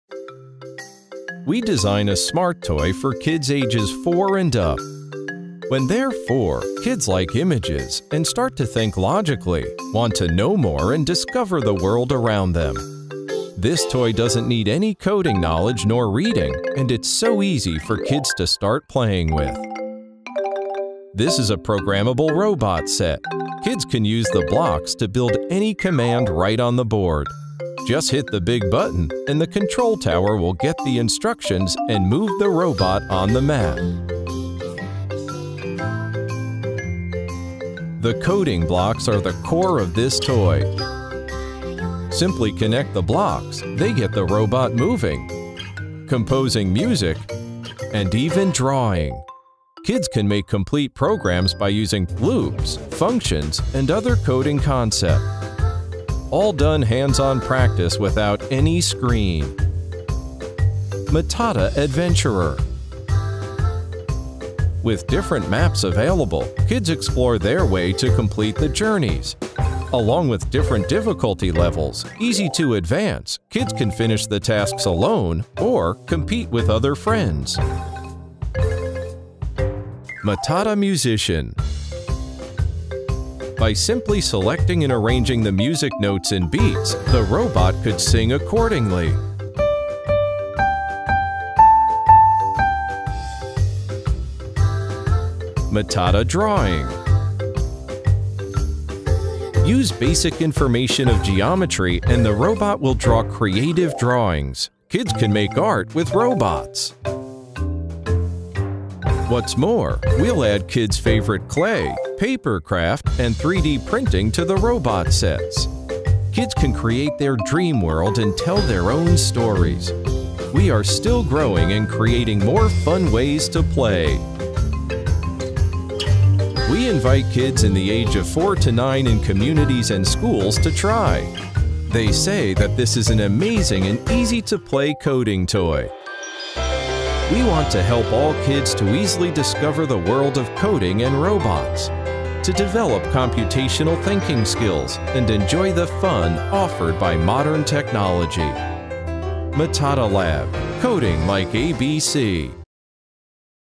Matatalab STEM Coding Set-A new hands-on coding robot for kids ages 4+
With simple logical games and the robot to make it fun and give instant feedback, coding no longer has to be difficult.